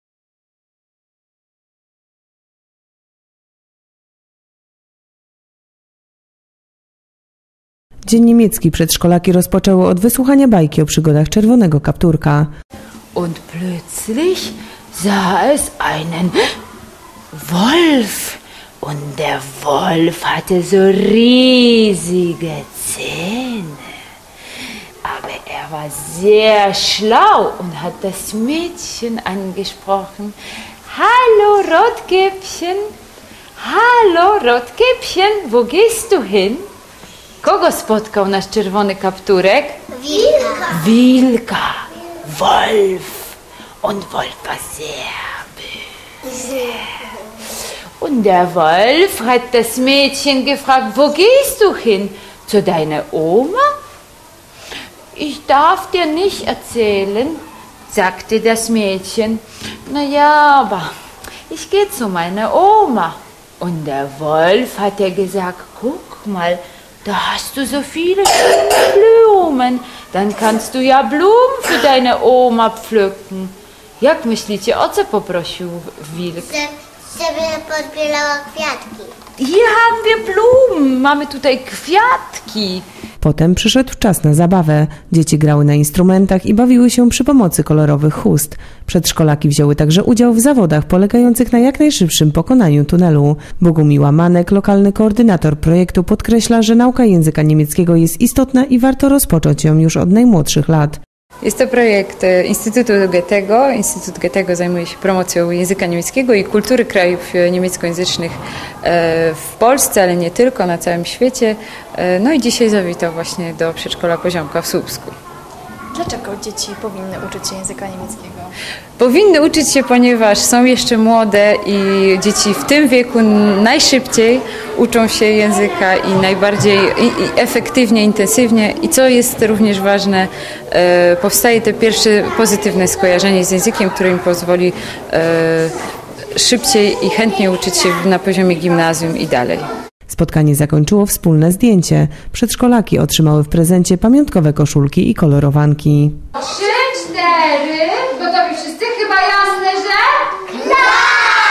DEUTSCH WAGEN TOUR
5 listopada nasze przedszkole odwiedziła przedstawicielka Instytutu Goethe i zaproponowała naszym maluchom szereg zabaw w języku niemieckim. Na początku spotkania to my jednak zaprezentowaliśmy naszemu gościowi swoje umiejętności językowe. Każda grupa przedszkolna zaśpiewała wybraną piosenkę i zadeklamowała jeden wierszyk w języku niemieckim.